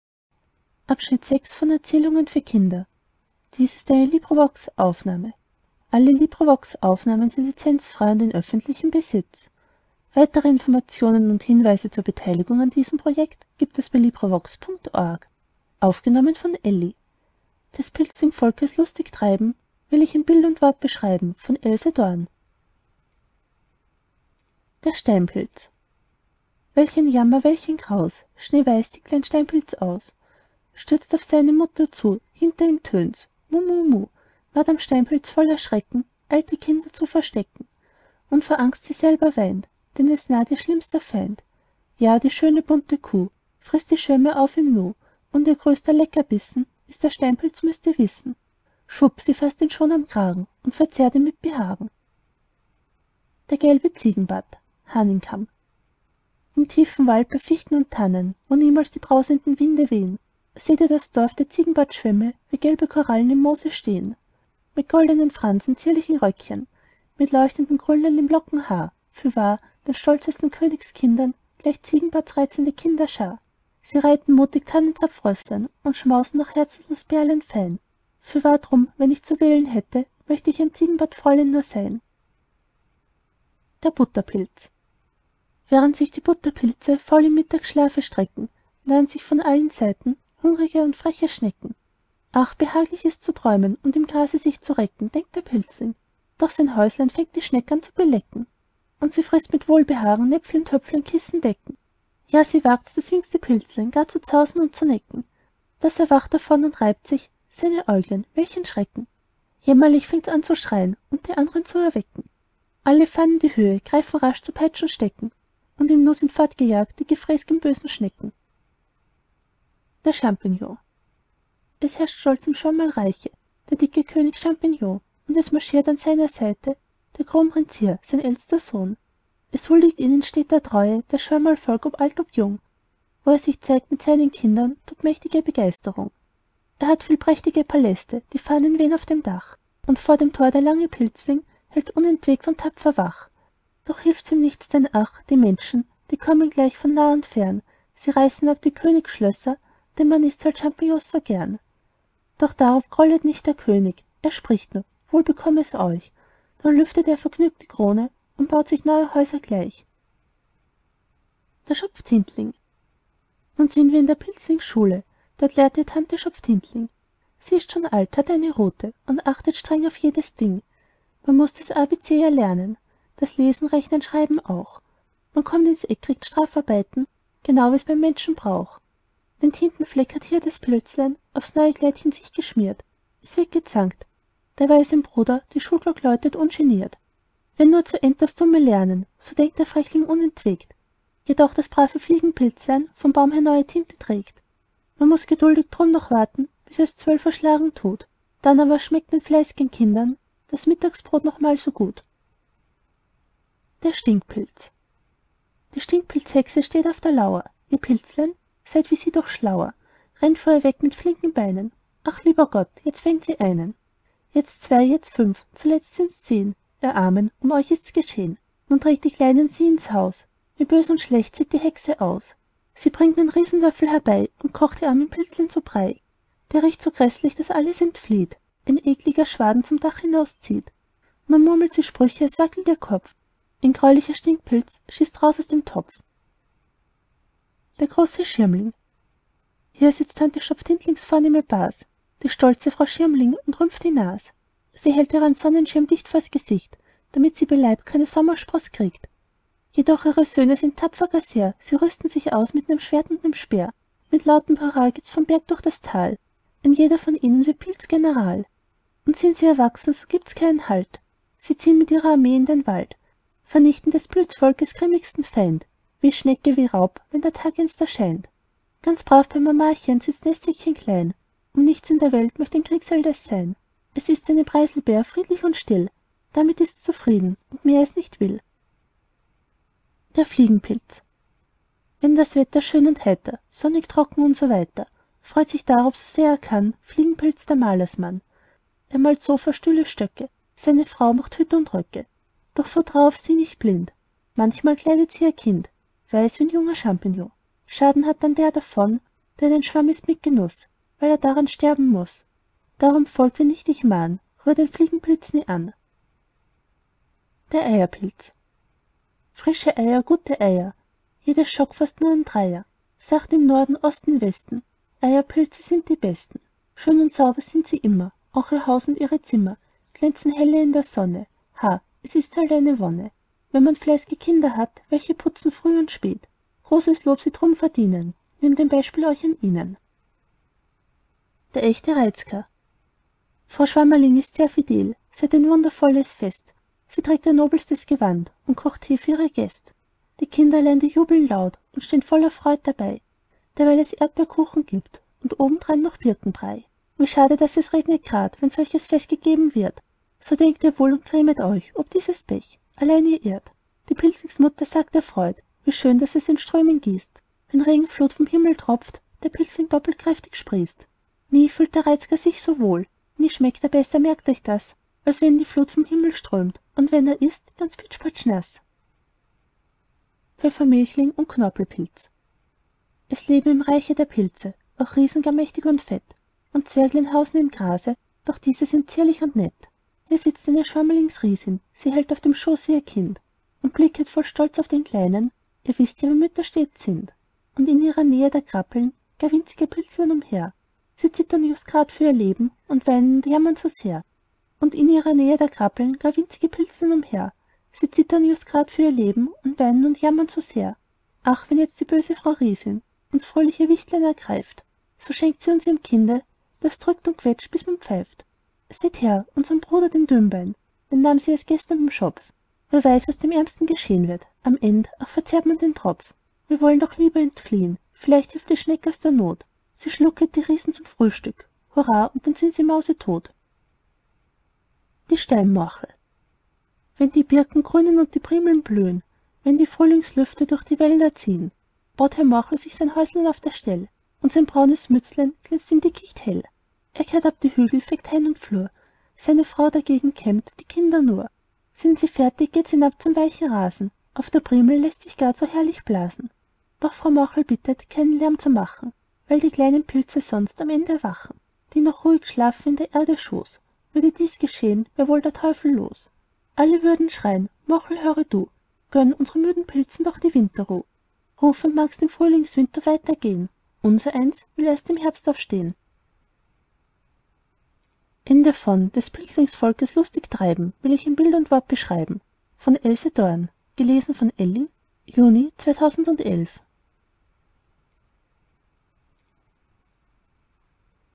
Des Pilzlingsvolkes lustig - Treiben will ich in Bild und Wort beschreiben. Hörbuch für Kinder
Else Forchheimer war eine Tochter des Holzhändlers und Sägewerksbesitzers Salomon Forchheimer und der Klara Ehrlich. Sie besuchte die höhere Töchterschule in Nürnberg und ein Töchterinstitut in Heidelberg. Sie heiratete 1898 den Nürnberger Rechtsanwalt Sigmund Dormitzer (1869–1943).

Dormitzer schrieb ab 1914 Feuilletons im Fränkischen Kurier. Sie arbeitete ab 1919 in der Verwaltung der jüdischen Kultusgemeinde in Nürnberg und wurde als erste Frau in den Hauptvorstand des Central-Vereins deutscher Staatsbürger jüdischen Glaubens berufen. Zwischen 1923 und 1927 veröffentlichte sie, vornehmlich unter dem Pseudonym Else Dorn, Kinderbücher in der Pestalozzi-Verlagsanstalt.

Das Ehepaar Dormitzer wurde in der Reichspogromnacht 1938 zweimal in ihrer Wohnung überfallen, misshandelt und verletzt. Ihr Haus und Grundstück mussten sie anschließend für 10 % des Wertes arisieren lassen und sie emigrierten am 1. März 1939 in die Niederlande und wohnten bei einer Tochter in Hilversum. Ihren Töchtern Elisabeth Rosenfelder und Hildegard Haas war vorher die Flucht nach Großbritannien und in die Niederlande gelungen. 1942 wurden sie aus Hilversum vertrieben und in Amsterdam ghettoisiert und am 22. April 1943 mit dem ersten Transport aus Holland in das Ghetto Theresienstadt deportiert. Ihr Ehemann starb am 9. Dezember 1943 in der Lagerhaft in Theresienstadt. Dormitzer wurde zu verschiedenen körperlichen Arbeiten herangezogen und engagierte sich im Kulturprogramm des Judenrats im Ghetto. Ab Oktober 1944 arbeitete sie in der Poststelle des Lagers. Nach der Befreiung durch die Rote Armee wurden die Niederländer, einschließlich der ehedem aus Deutschland nach Holland emigrierten Deutschen im Juni 1945 mit Autos aus Theresienstadt nach Bamberg und von dort in Viehwaggons in die Niederlande transportiert. Die Emigranten wurde im Quarantänelager Sittard interniert und im Juli 1945 entlassen.

Dormitzer übersiedelte nach England und erhielt 1951 die britische Staatsbürgerschaft. Dormitzer berichtete nach Kriegsende in drei Folgen über Theresienstadt und hielt auch Rundfunkvorträge im BBC. Ihre in der Haft geschriebenen Gedichte konnte sie 1945 in Hilversum veröffentlichen.

Ihre Manuskripte gingen bei der Flucht aus dem nationalsozialistischen Deutschland verloren, ihre Tagebuchaufzeichnungen wurden auf ihren eigenen Wunsch nach ihrem Tod vernichtet.